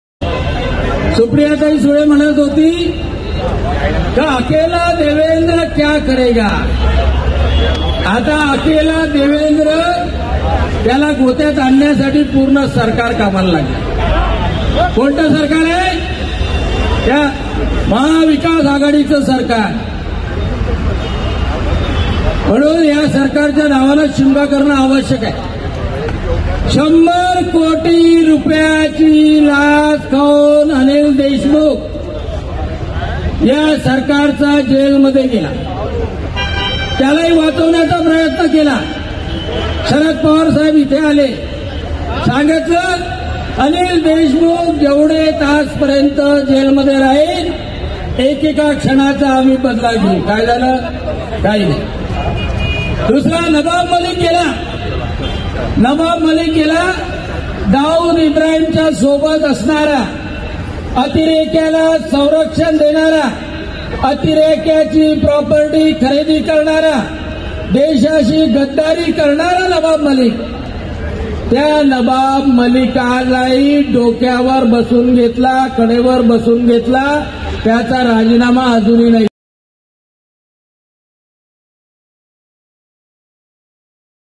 राष्ट्रवादीचे दोन मंत्री जेल मध्ये आणि आणखी जाण्याच्या तयारीत आहे.- अनिल बोंडे
अकेला देवेंद्र क्या कर लेगा..? दोन मंत्री जेल मध्ये आणि आणखी जाण्याच्या तयारीत आहे.
पूर्ण राष्ट्रवादी कामाला लावली आणि  सुप्रिया सुळे म्हणतात "अकेला देवेंद्र क्या कर लेगा...
- डॉ. अनिल बोंडे (भाजपा नेते)